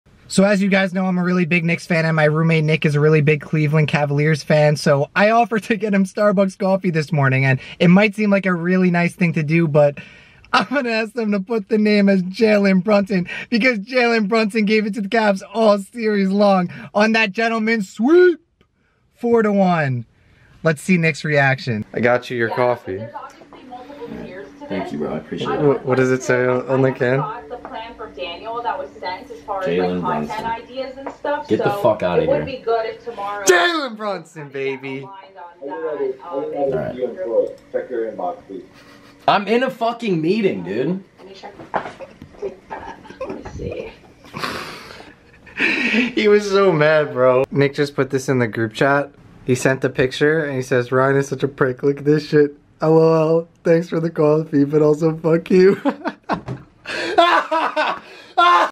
Getting A Cavs Fan A Jalen Brunson Coffee
Trolling My Roommate who is a Cleveland Cavaliers Fan After The Knicks Beat the Cavaliers

USE CODE "thatnykid" IN THE ITEM SHOP!